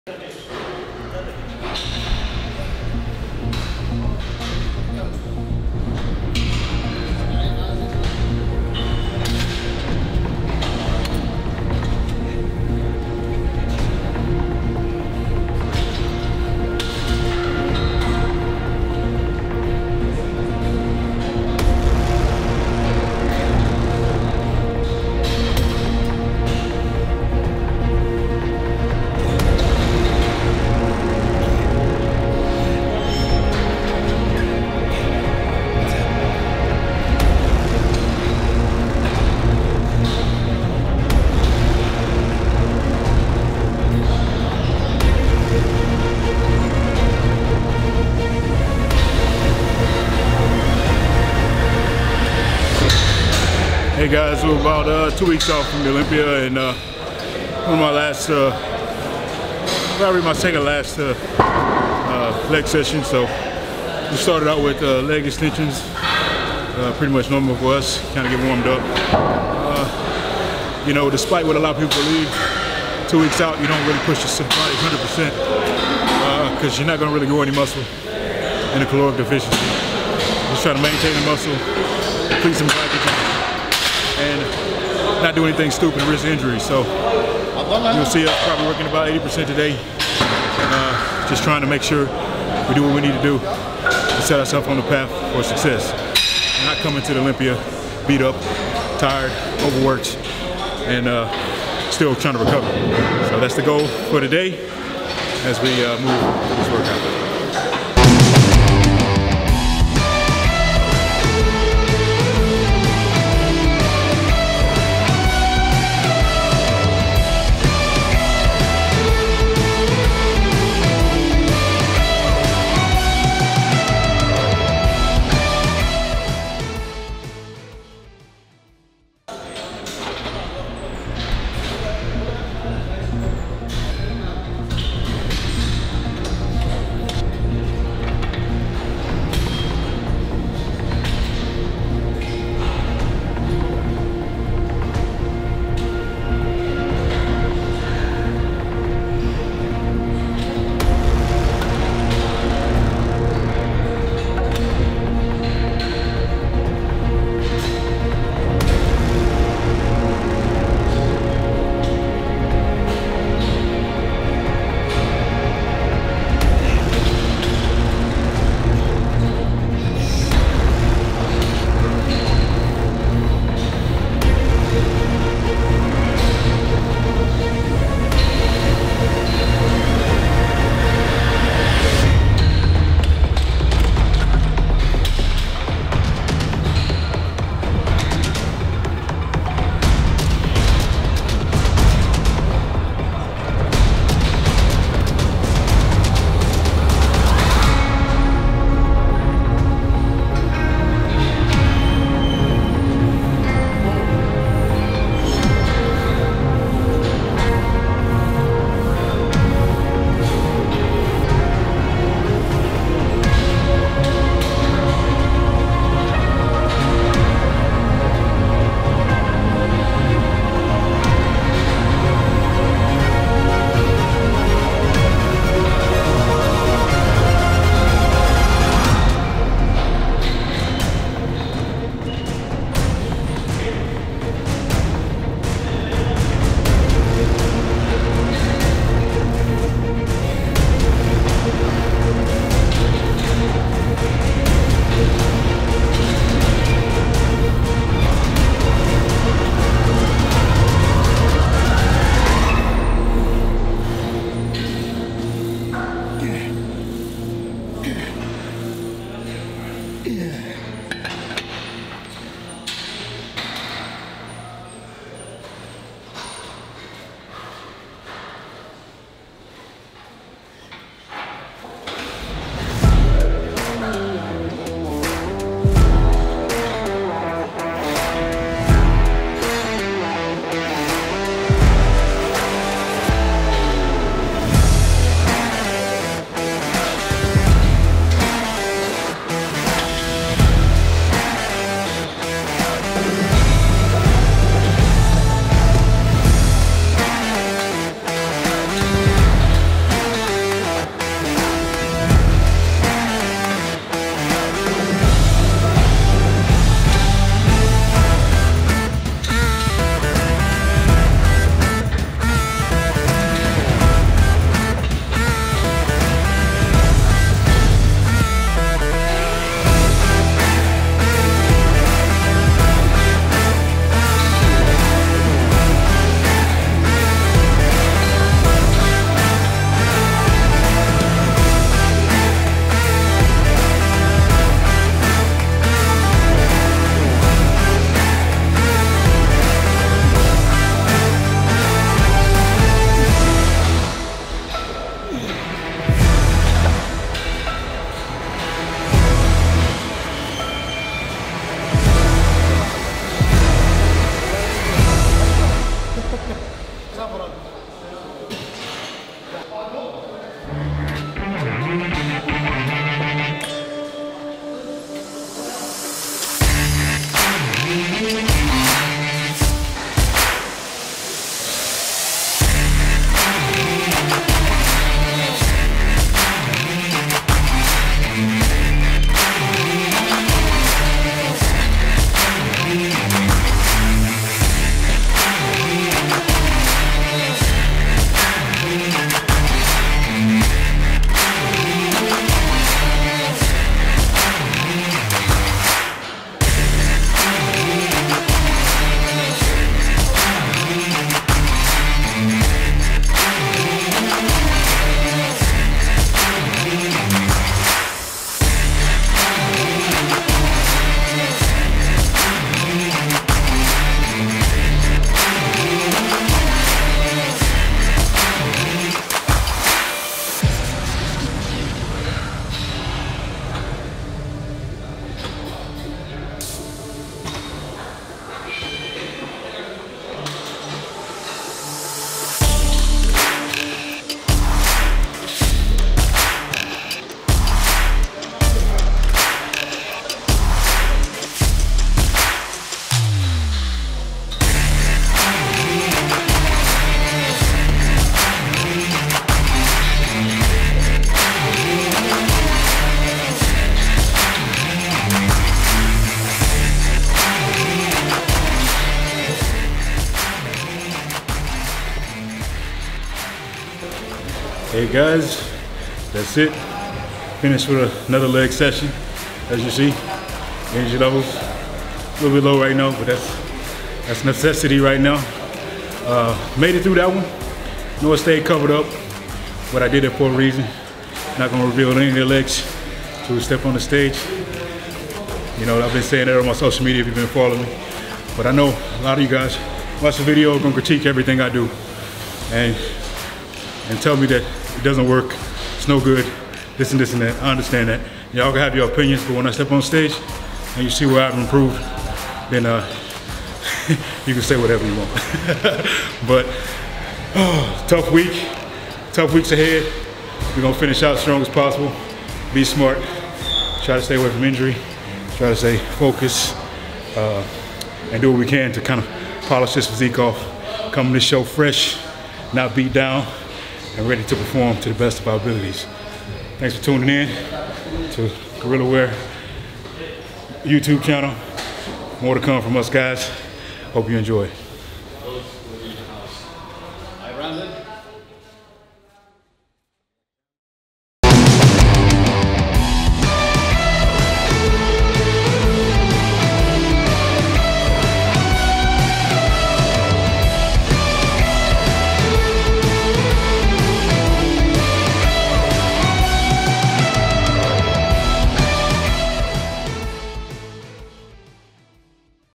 Leg Day | Brandon Curry | 2 Weeks out | Road 2 Olympia 2020
In this video, we follow Gorilla Wear Athlete Brandon Curry in his preparation for the 2020 Mister Olympia. Brandon Curry in this video is just under 2 weeks out from 2020 Mister Olympia competition. See how he trains legs together with his training partner and coach Abdullah.